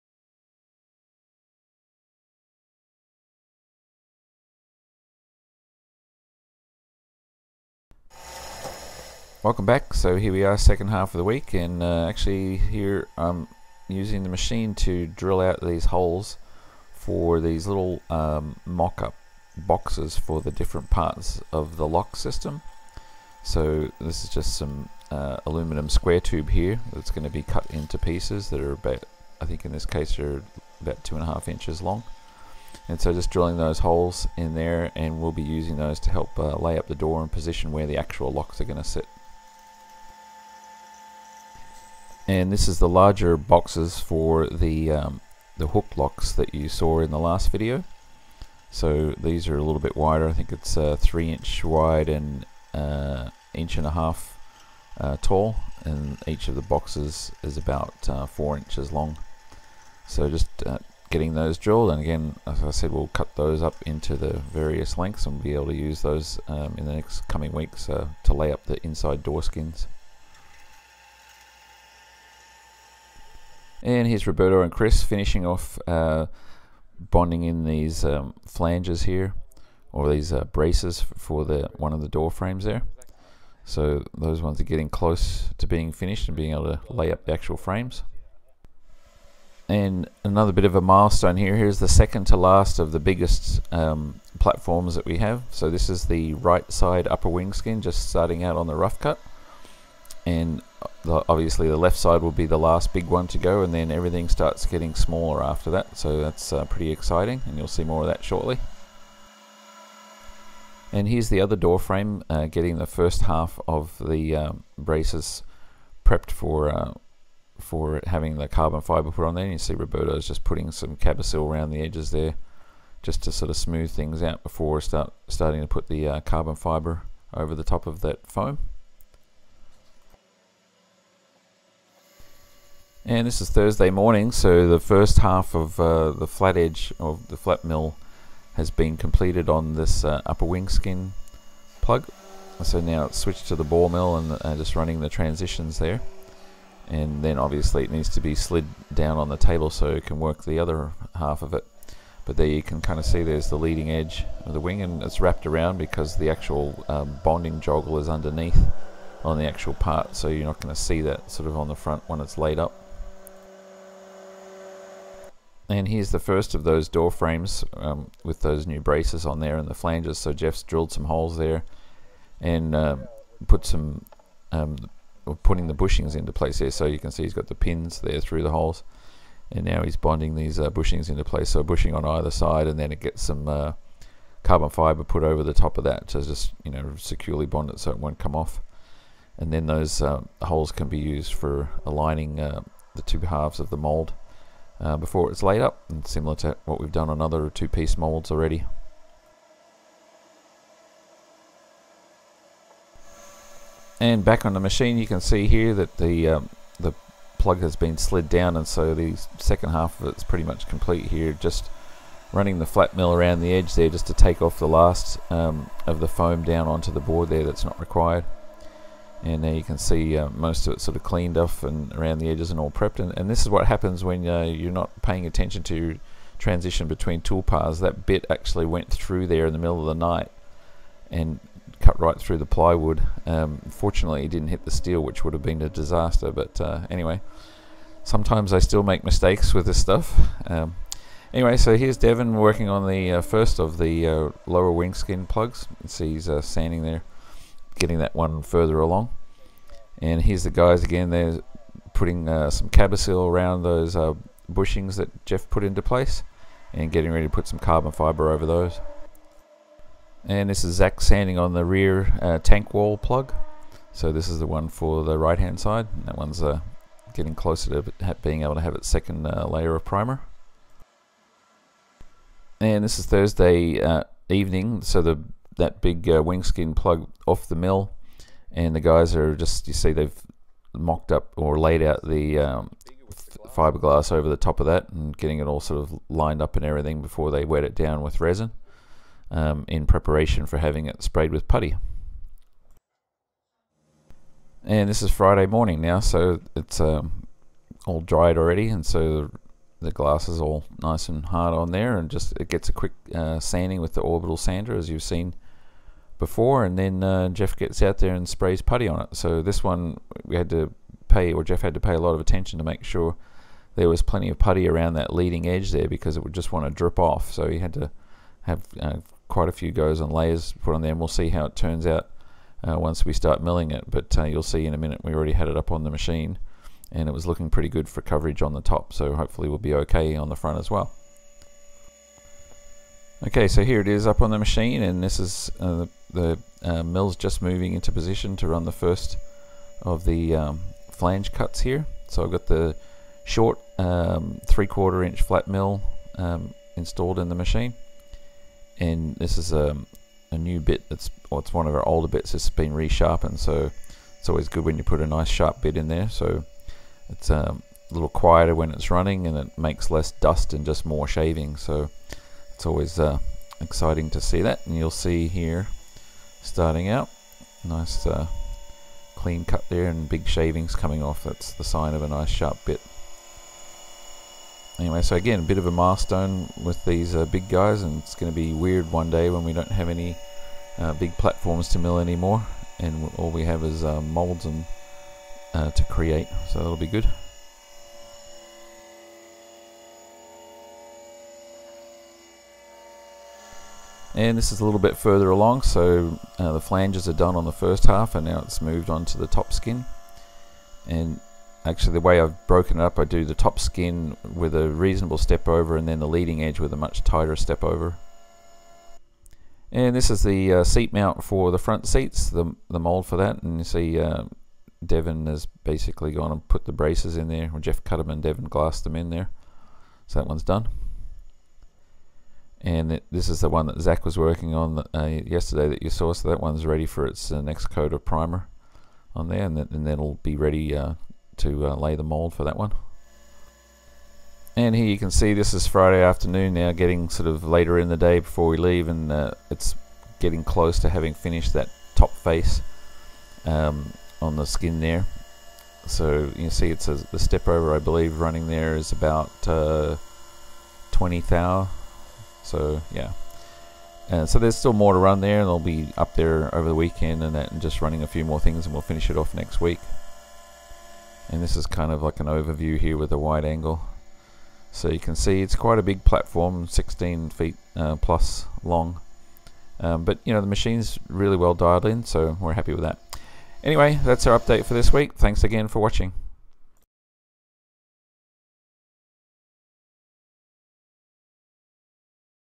Raptor Aircraft November 19th 2016
Milling the third of the large wing skin plugs.